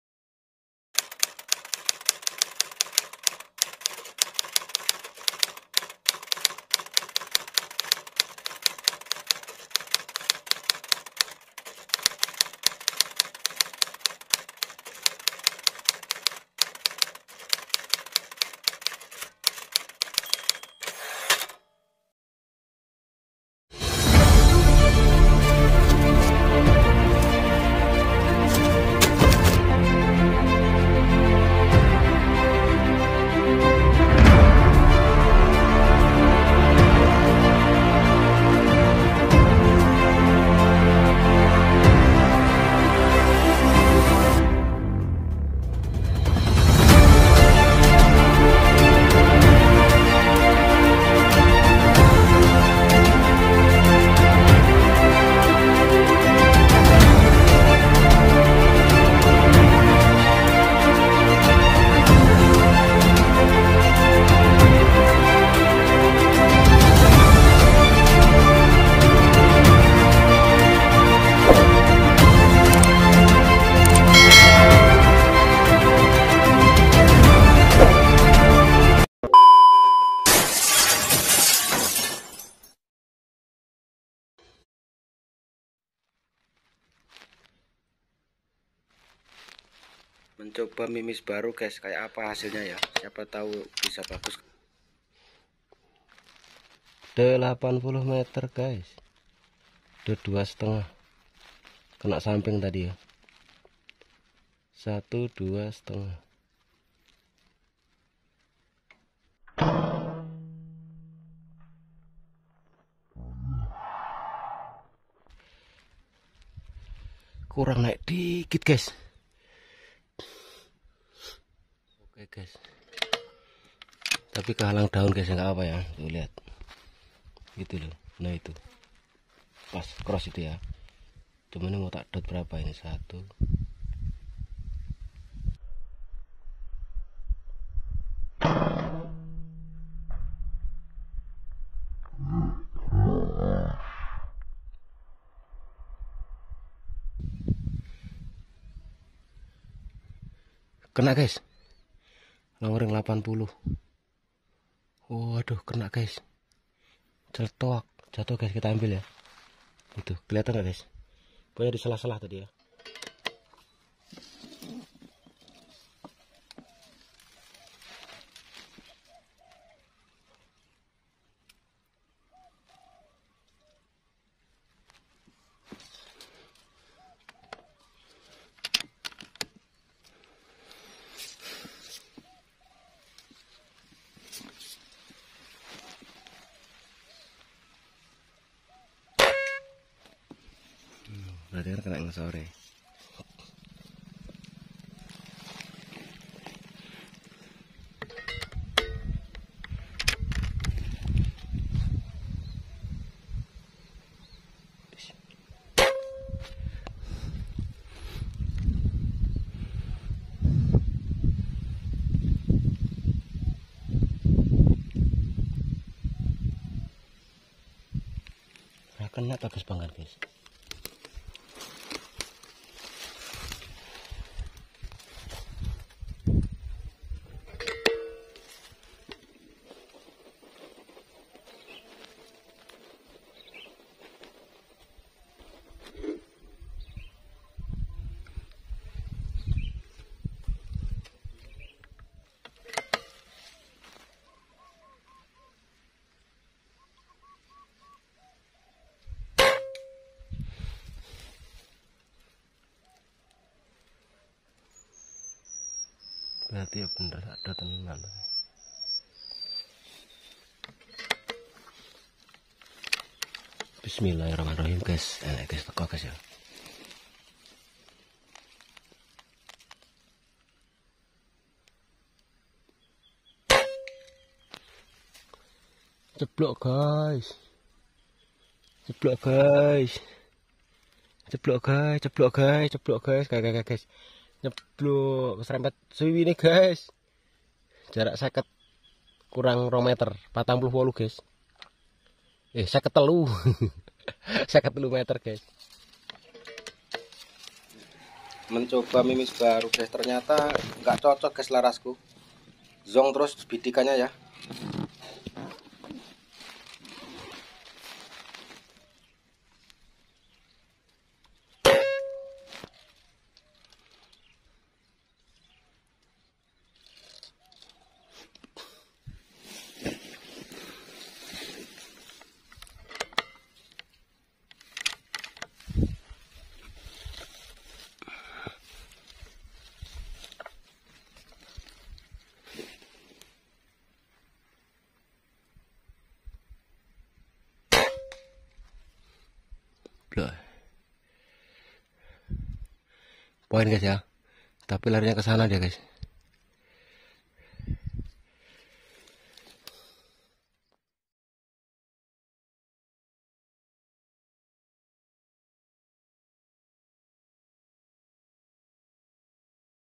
berburu kesalahan dalam memilih mimis apa yang terjadi
kata hati lagi memilih mimis yang cocok tapi malah hasil nya minim dan tidak memuaskan hasil bidikan nya, maka dari itu sebaik apapun Laras nya Tp kalau salah memilih mimis akan terjadi kesalahan dalam membidik